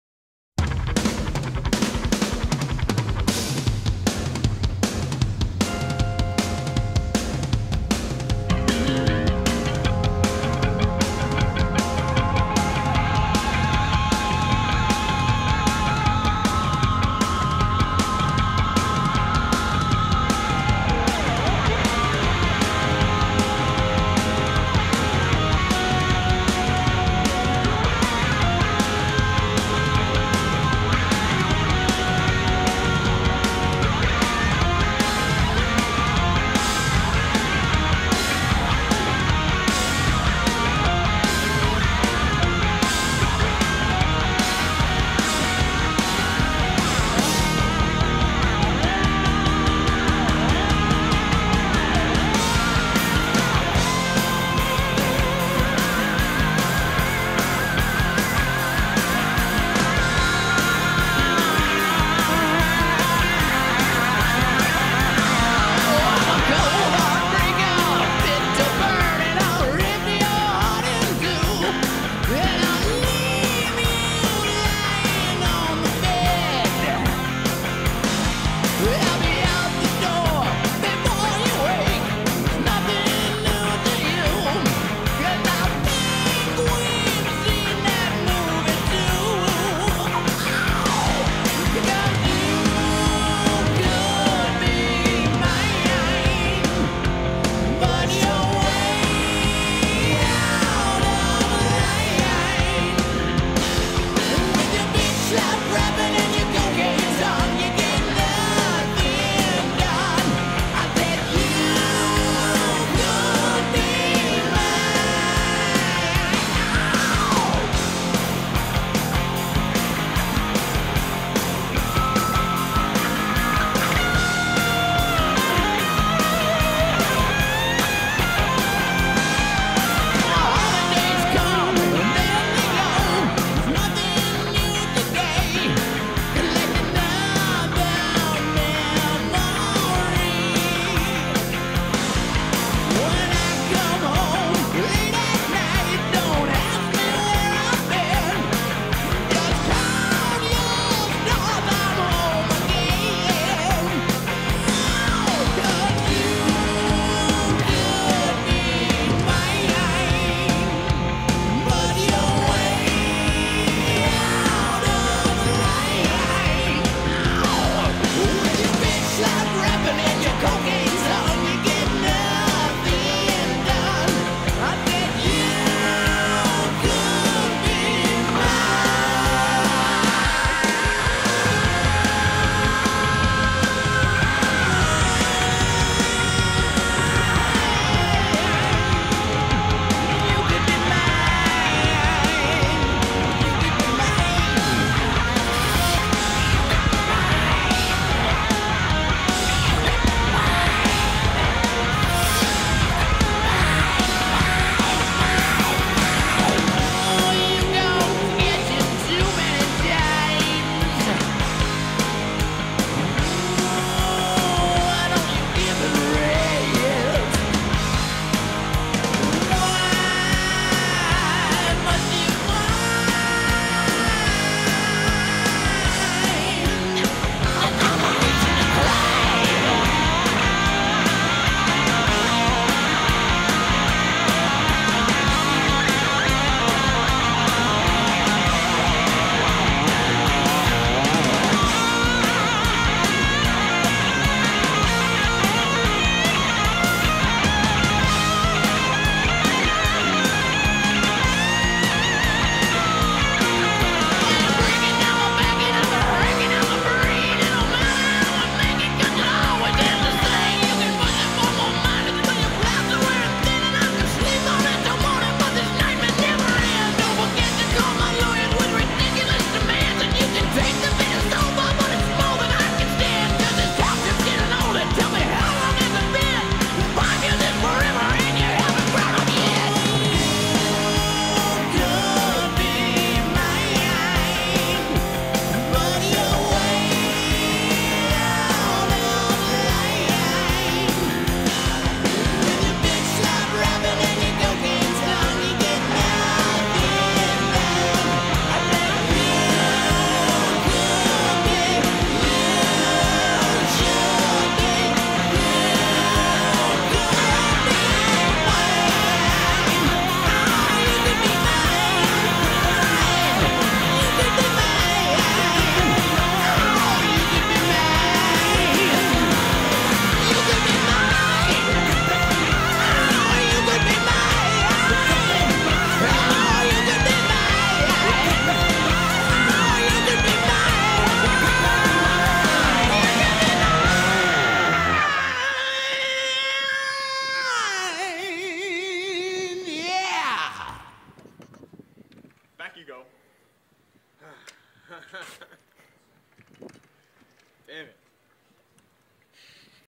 Speed Chess
I am playing chess with a friend of mine. In this game I give him five minutes for the whole game and I take 1 minute for the whole game. I by no means own the rights to Guns and Roses music, it is here solely for entertainment. Other than that, sit back, relax and enjoy the video! By the way, this is NOT sped up at all. The only thing I added in this video was the music.